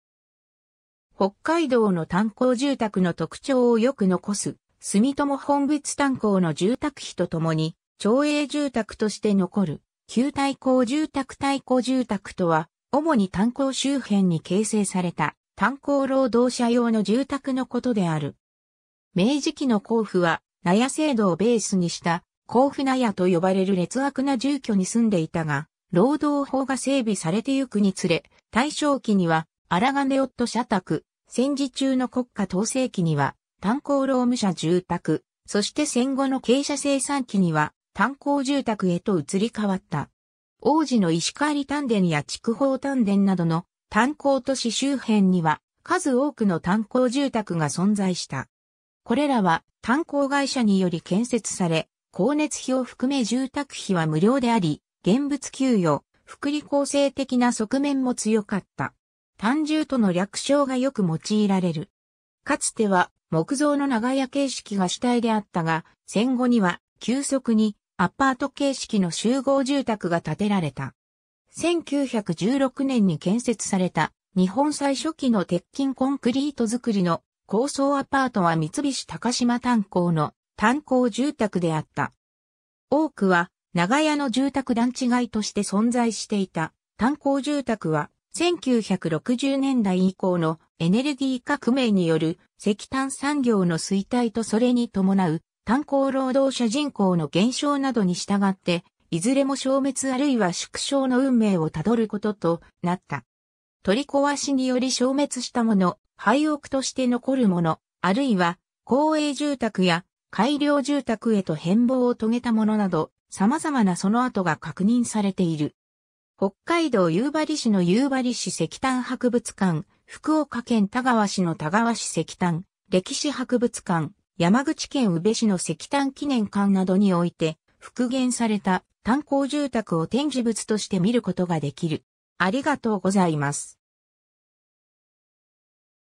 炭鉱住宅
北海道の炭鉱住宅の特徴をよく残す住友奔別炭鉱の住宅 碑とともに町営住宅として残る旧炭鉱住宅（福岡県嘉麻市、漆生炭坑跡） 炭鉱住宅（たんこうじゅうたく）とは、主に炭鉱周辺に形成された、炭鉱労働者用の住宅のことである。
 明治期の鉱夫は納屋制度をベースにした「坑夫納屋」と呼ばれる劣悪な住居に住んでいたが、労働法が整備されてゆくにつれ、大正期には「鑛夫社宅」、戦時中の国家統制期には「炭鉱労務者住宅」、そして戦後の傾斜生産期には「炭鉱住宅」へと移り変わった。
 往時の石狩炭田や筑豊炭田などの炭鉱都市周辺には数多くの炭鉱住宅が存在した。
これらは炭鉱会社により建設され、光熱費を含め住宅費は無料であり、現物給与・福利厚生的な側面も強かった。
炭住（たんじゅう）との略称がよく用いられる。
 かつては木造の長屋形式が主体であったが、戦後には急速にアパート形式の集合住宅が建てられた。
1916年（大正5年）に建設された日本最初期の鉄筋コンクリート造の高層アパートは三菱高島炭鉱（軍艦島）の炭鉱住宅であった。
 多くは長屋の住宅団地街として存在していた炭鉱住宅は、1960年代以降のエネルギー革命による石炭産業の衰退とそれにともなう炭鉱労働者人口の減少などにしたがって、いずれも消滅あるいは縮小の運命を辿ることとなった。
取り壊しにより消滅したもの、廃屋として残るもの、あるいは公営住宅や改良住宅へと変貌を遂げたものなど、様々なその後が確認されている。
 北海道夕張市の夕張市石炭博物館、福岡県田川市の田川市石炭・歴史博物館、山口県宇部市の石炭記念館などにおいて、復元された炭鉱住宅を展示物として見ることができる。